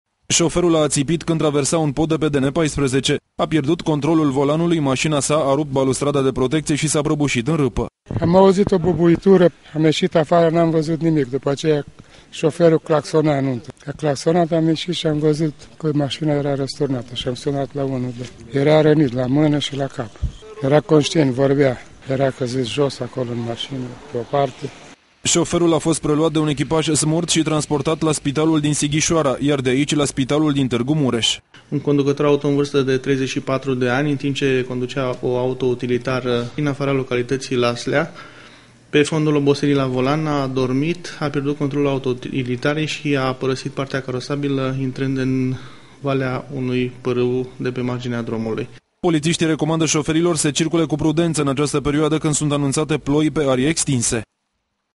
A CAZUT IN GOL
La un pas de moarte ! Soferul unei autoutilitare a atipit  la volan  si    a cazut cinci metri in  gol de pe un  pod.  Accidentul s-a produs pe  DN  14 in apropiere de  localitatea Laslea. Conducatorul auto s-a ales cu multiple  traumatisme si fracturi dar a scapat cu viata.